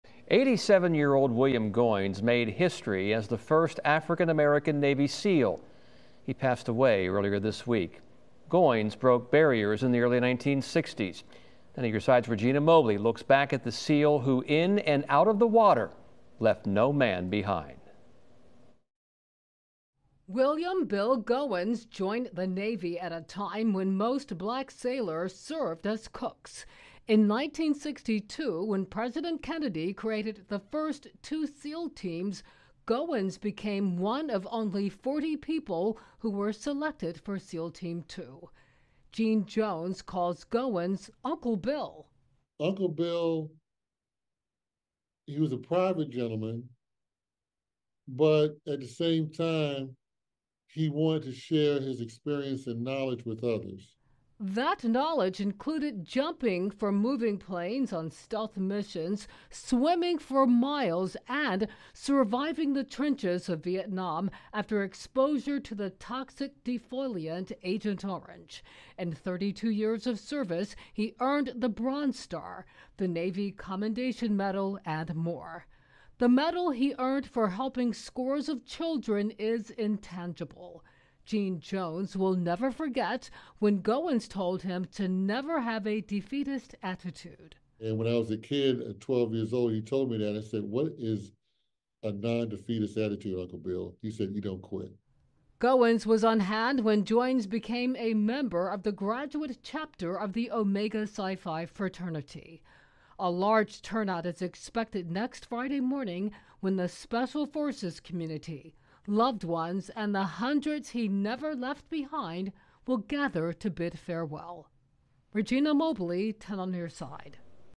America's first Black Navy SEAL dies at 87
William Harvey Goines, America's first Black Navy SEAL, died at the age of 87 on Monday, June 10. His service will be held in Norfolk later this month.